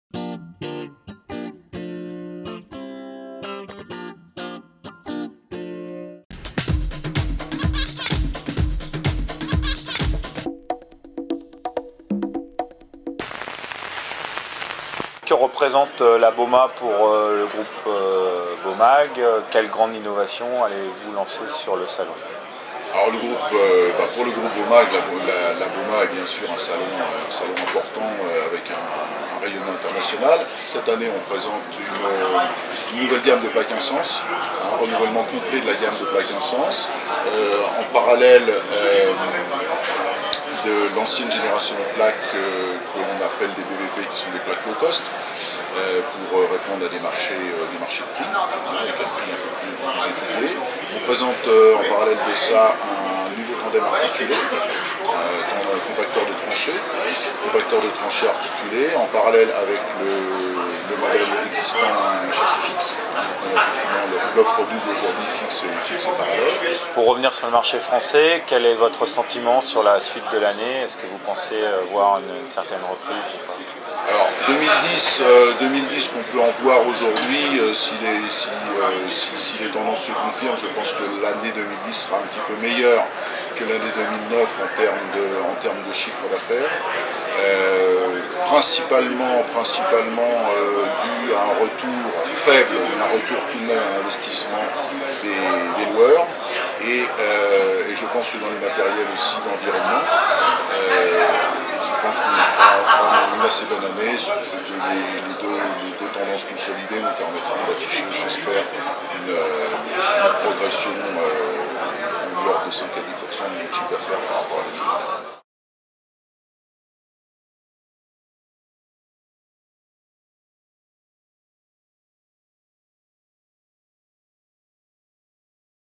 BAUMA 2010 : Philippe Ducouret, directeur général de Bomag France
Philippe Ducouret, directeur général de Bomag France rappelle l'importance du salon Bauma pour le groupe allemand sur le plan international.